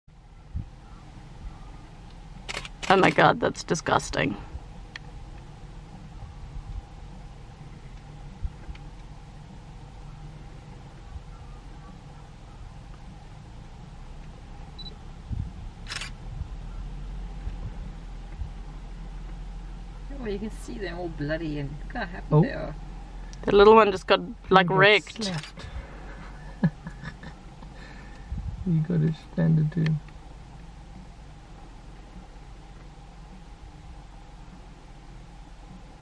Crocodile eating water buck one hits another with tail (warning sensitive viewers)
Kruger National Park sighting by D.n.Adventure outside of Shingwedzi Rest Camp.